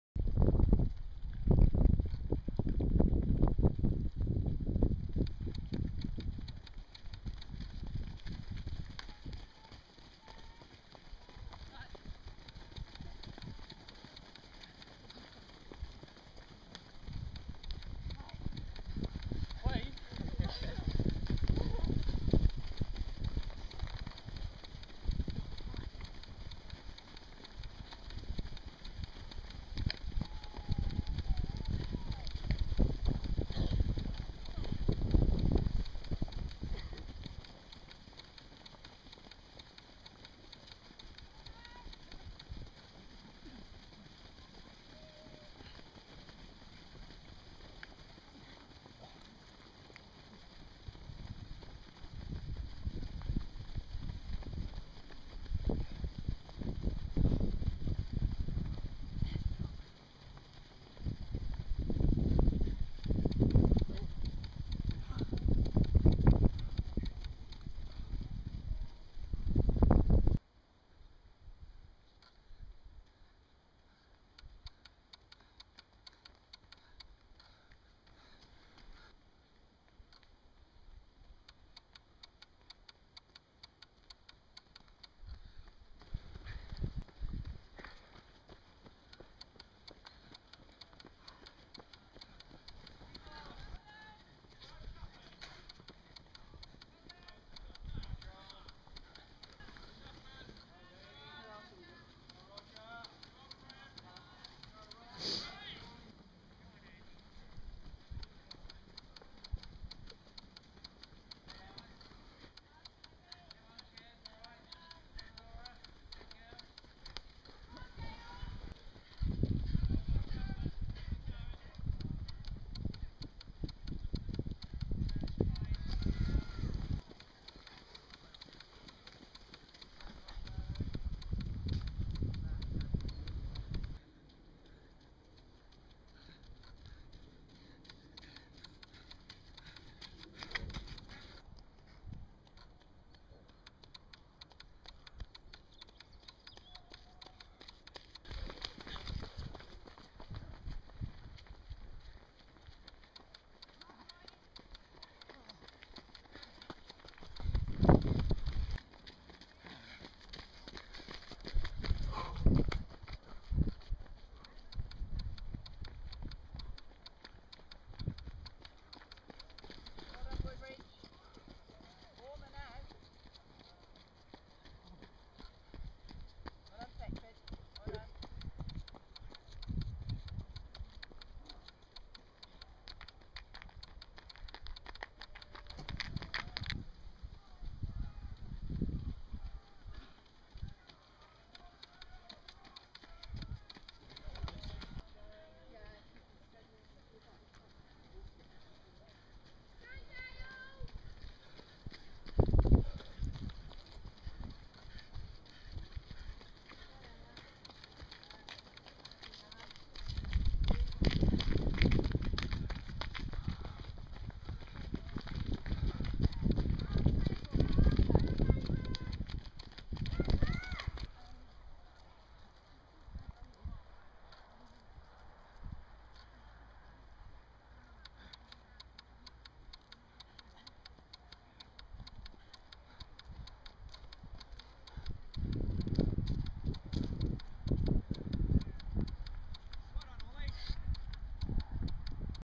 Ipswich Harriers cross country - 2
Ipswich Harriers at the Suffolk Winter League cross country at Sutton, Woodbridge, Suffolk on 4 December 2011